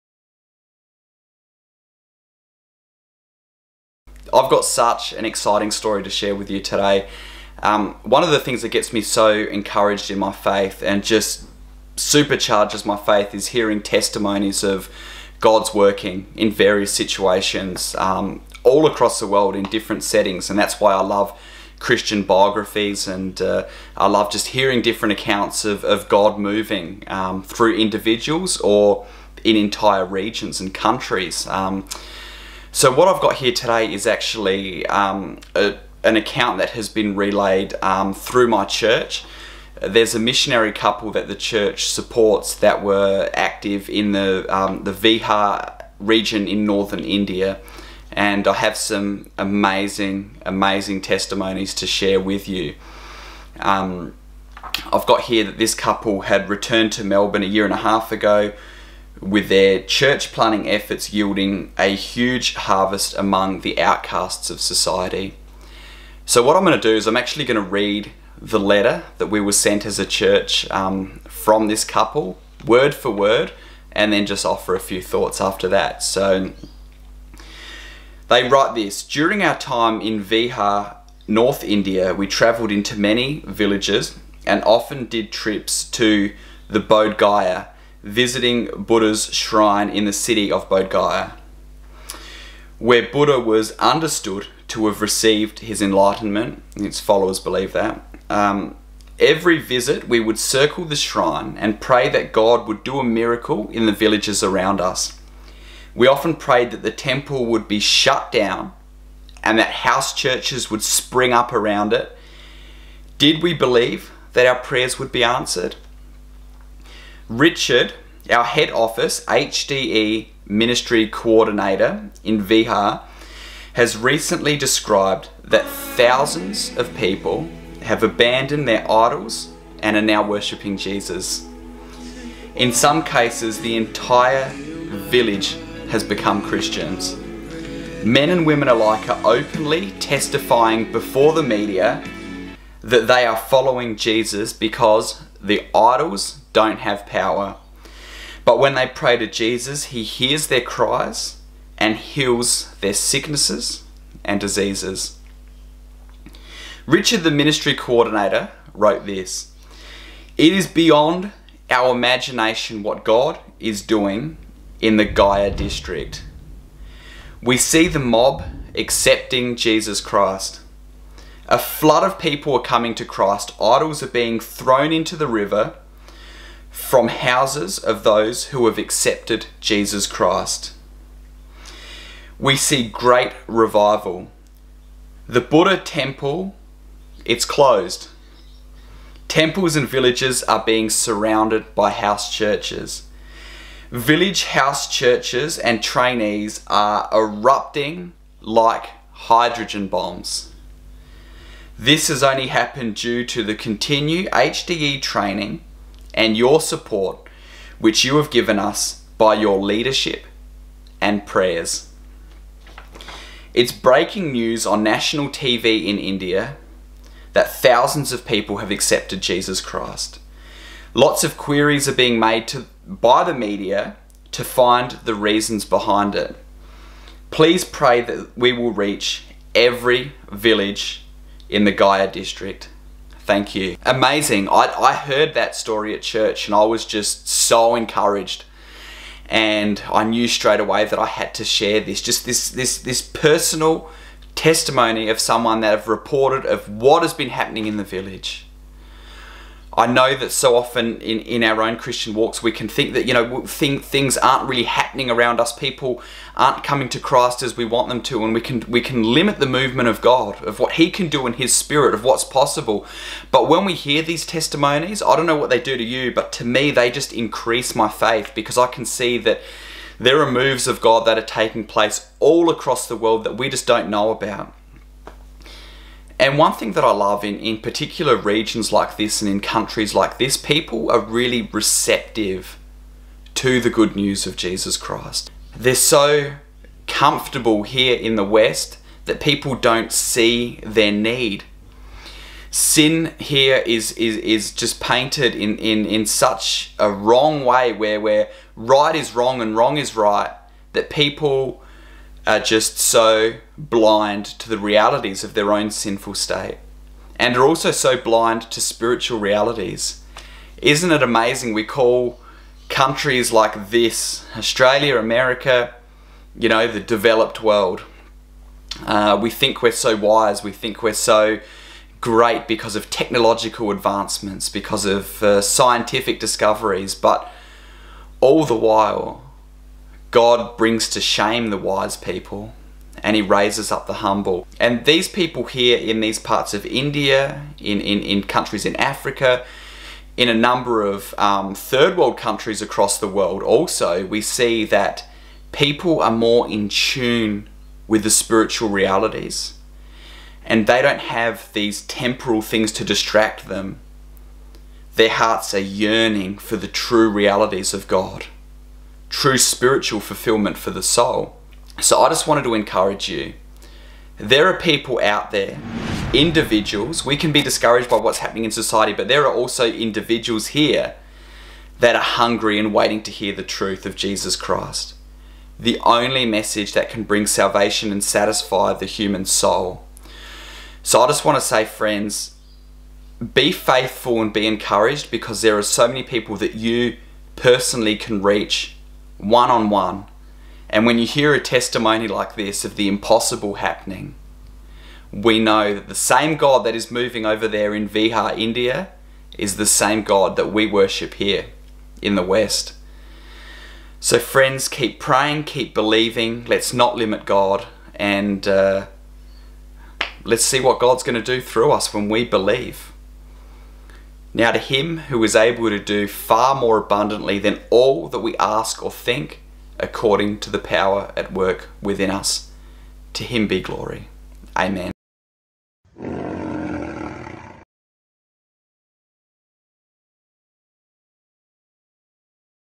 MASSIVE REVIVAL 😮🤭 | National Media CANNOT EXPLAIN This!! 😮🤭 | Movement of the Holy Spirit
This story of a MASSIVE REVIVAL in India is incredible. The national media cannot explain this movement of the Holy Spirit! This is a huge revival, a huge move of God, with thousands coming to Christ and forsaking idols for Christ.